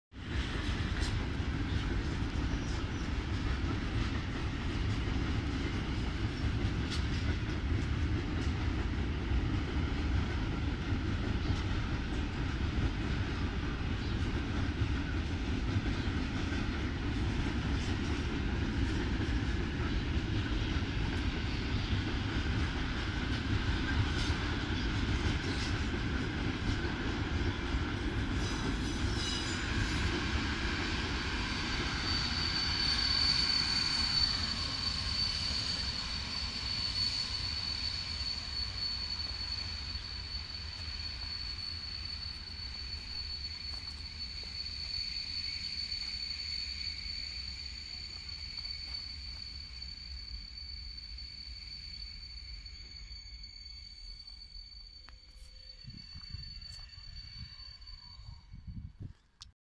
wagony bez lokomotywy same jadą (widmo???) | Lubin
Oczywiście była lokomotywa tylko teamt filmu